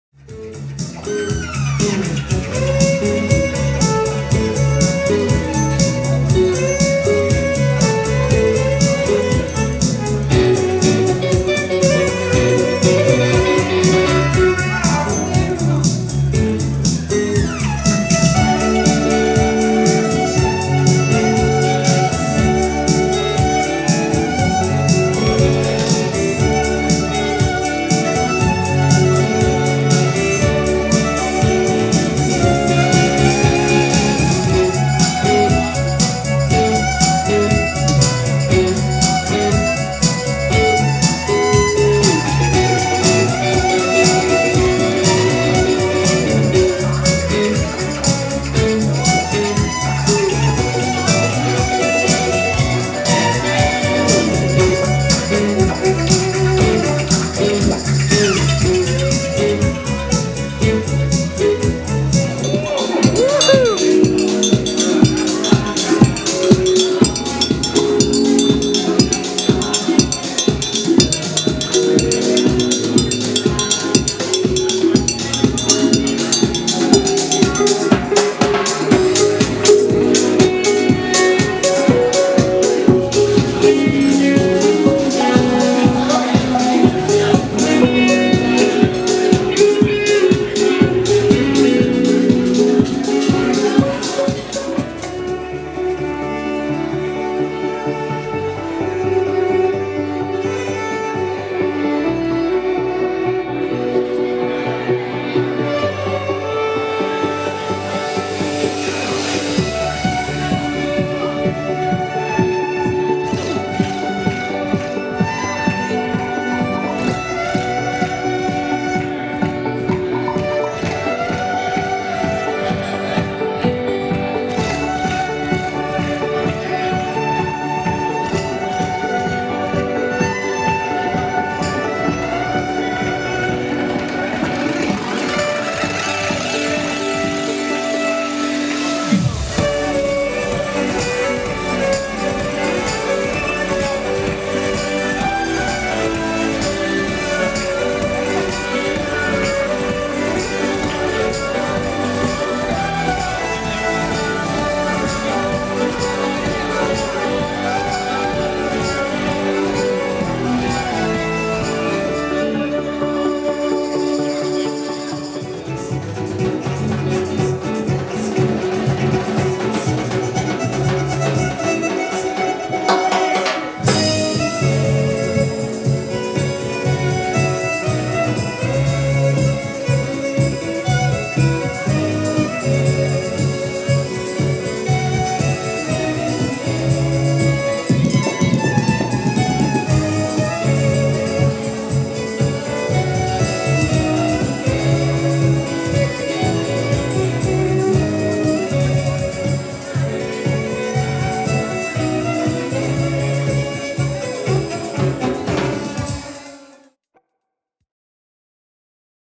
Luciano Pavan Band: Violin/ DJ/ Percussion/ Guitar - Live in Pueblo Bonito
LP Band/ Live in Pueblo Bonito, Los Cabos, Mexico

Luciano Pavan: Electric Violin
Diego Sole: Guitar
Jerry: Percusion
Marck: DJ